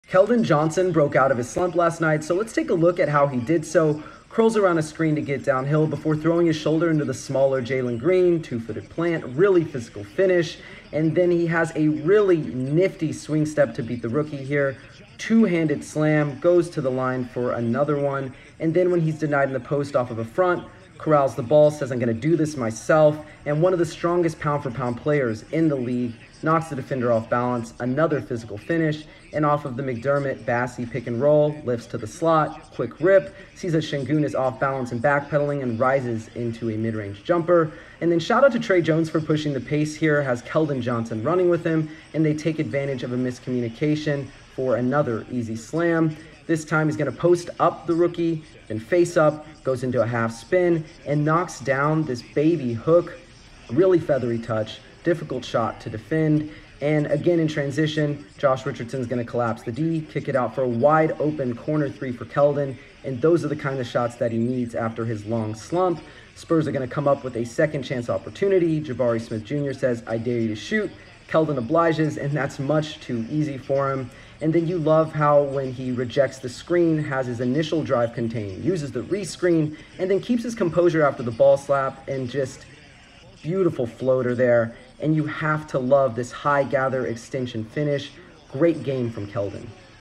Spurs Film Study: Keldon Johnson Snaps Slump
Breaking down how Keldon Johnson snapped his shooting slump against the Houston Rockets.

@N_Magaro and @poundingtherock